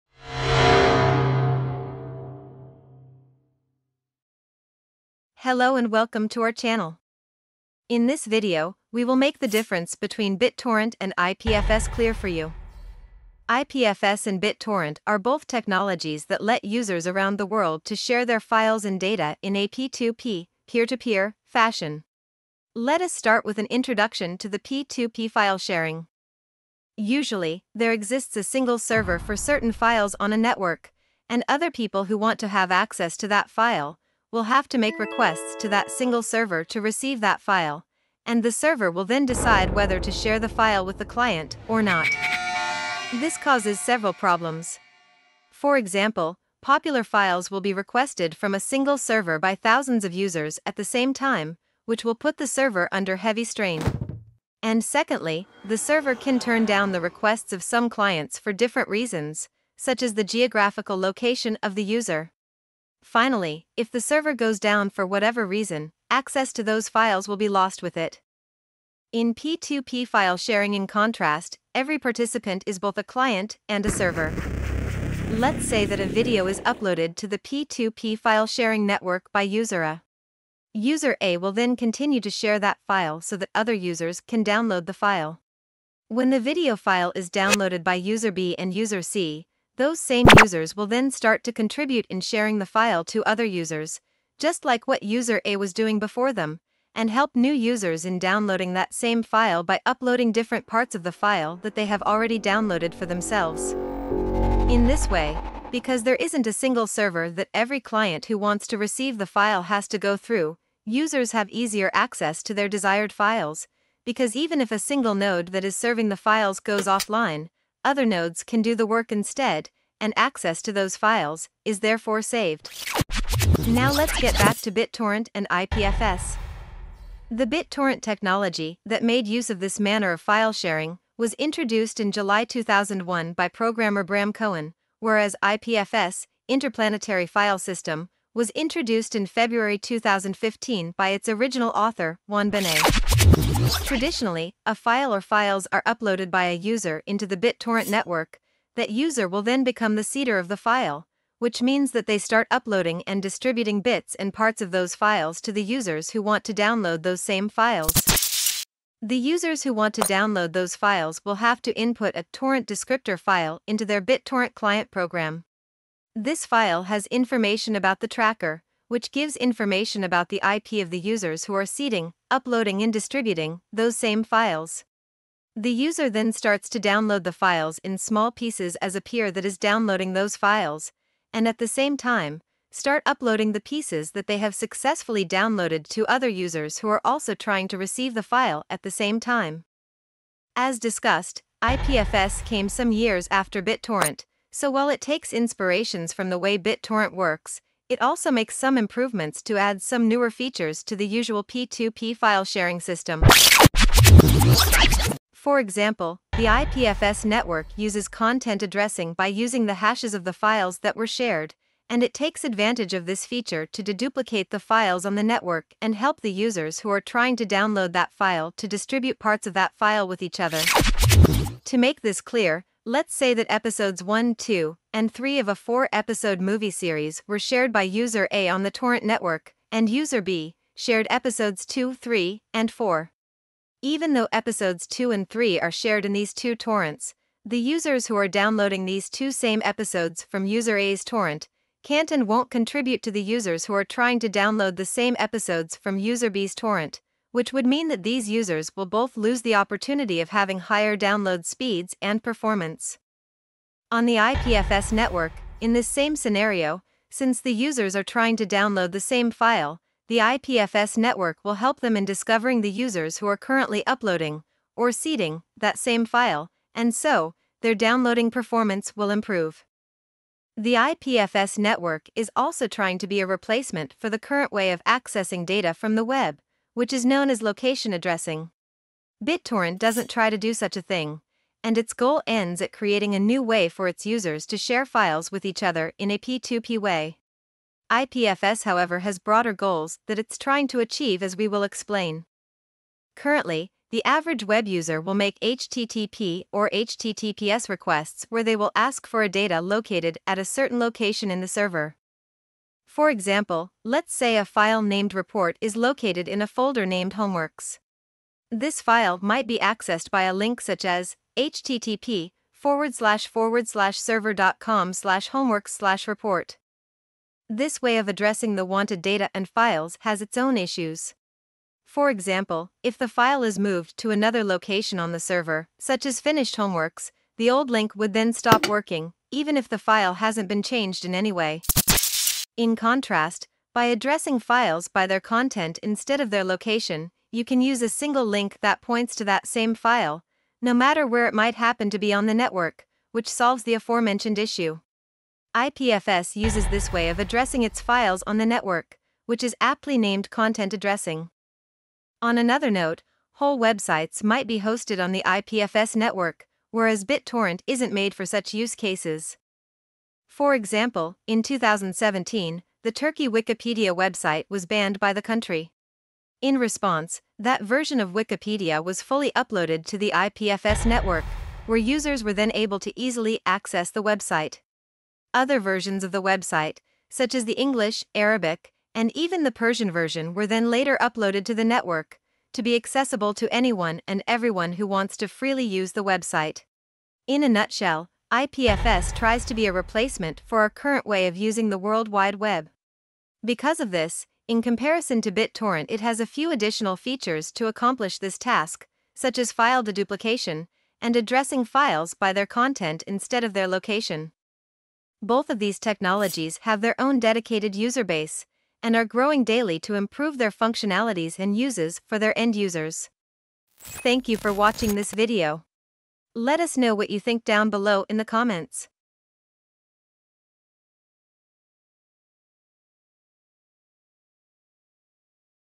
BitTorrent vs. IPFS
In this video we will touch on the differences between BitTorrent and IPFS. As you will see, despite having their similarities, some crucial differences set these two technologies apart!